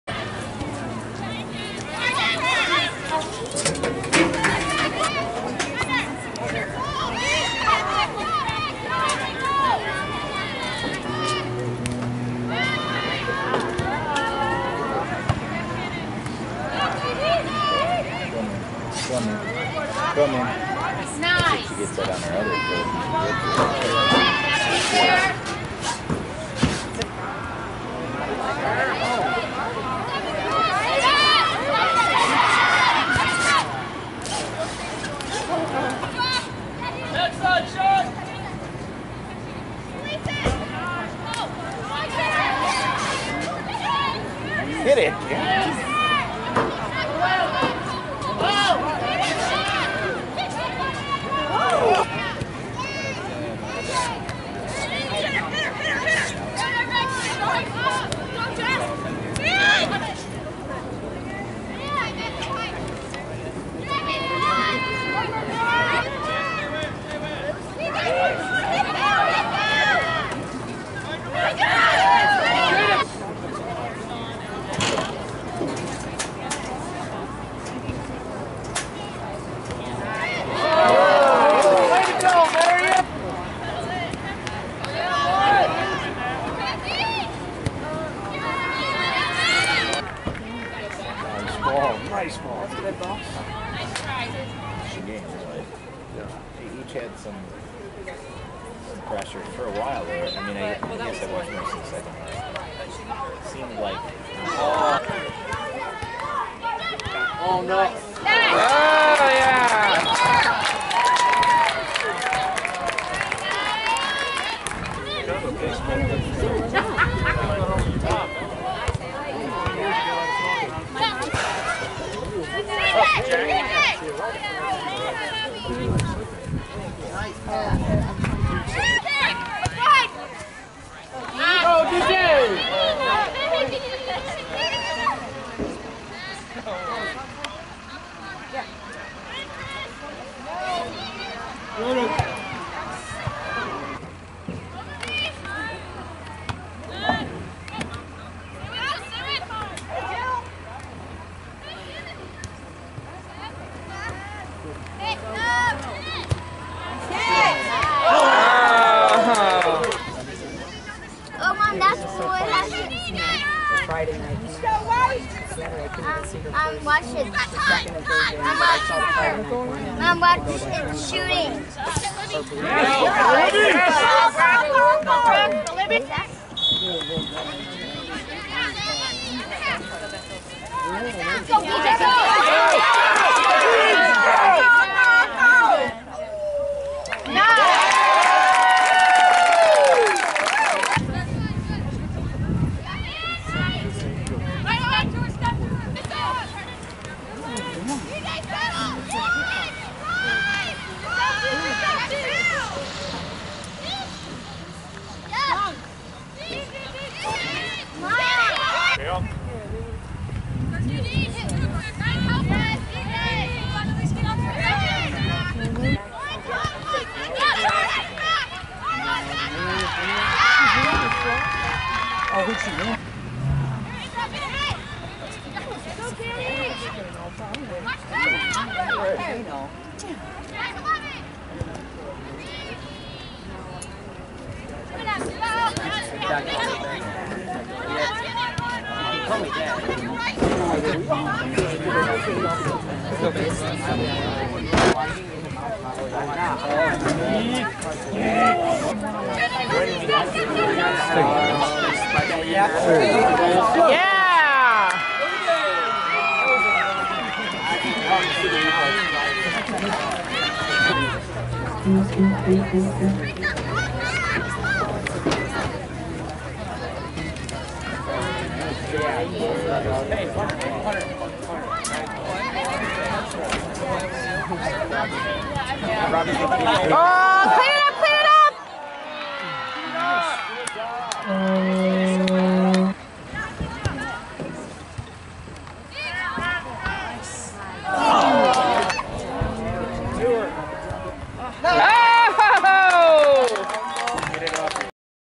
Denise Veldman Soccer highlights
Denise Veldman (DJ) Soccer Highlights Class of 2015
Marian High School Girls Soccer Team
# 14    Jersey/short Colors - Blue/Blue or White/White